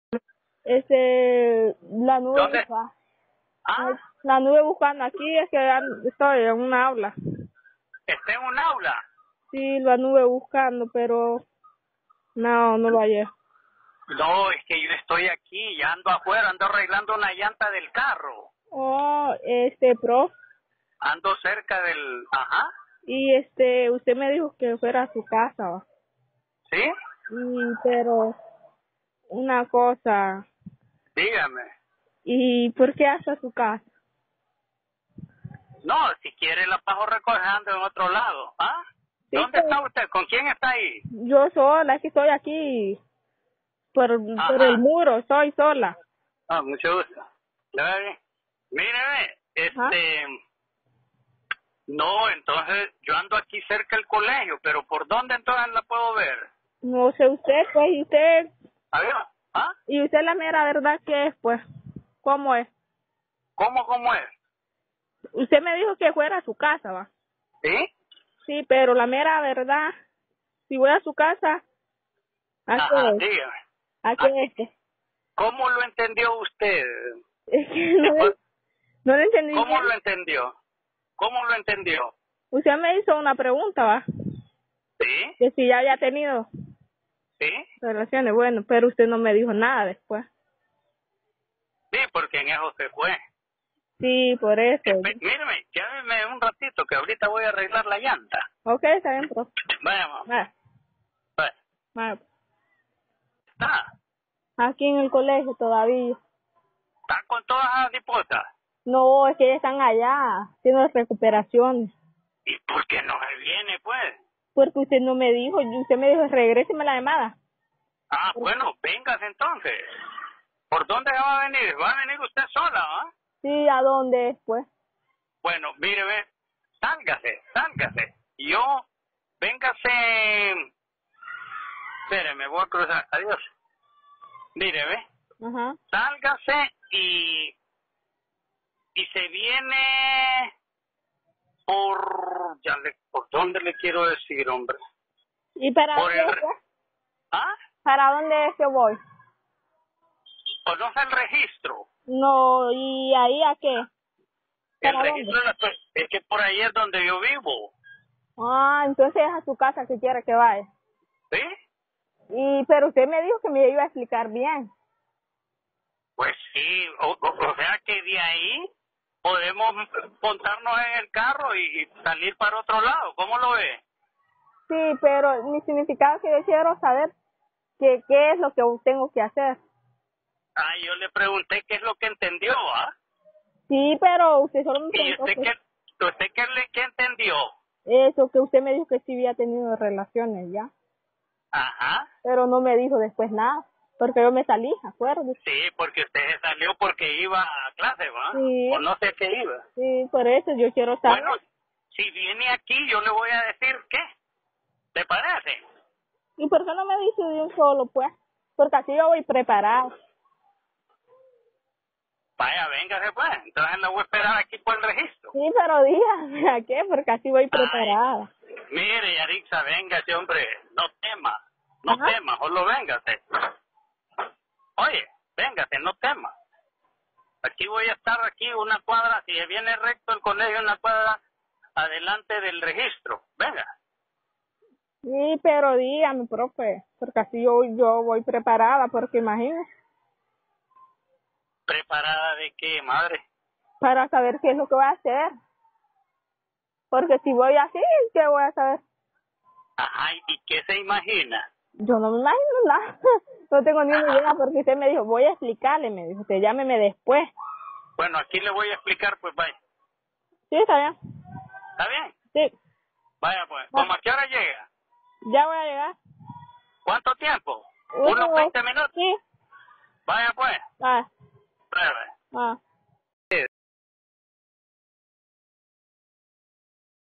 Honduras publican audio donde maestro acosa sexualmente alumna
Un escándalo ha estallado entre los padres de familia de un colegio en Honduras, al trascender un audio en donde un maestro intenta seducir a una alumna.
El hecho ocurrió en el Instituto Terencio Sierra de la ciudad de Nacaome, departamento de Valle, zona sur de Honduras.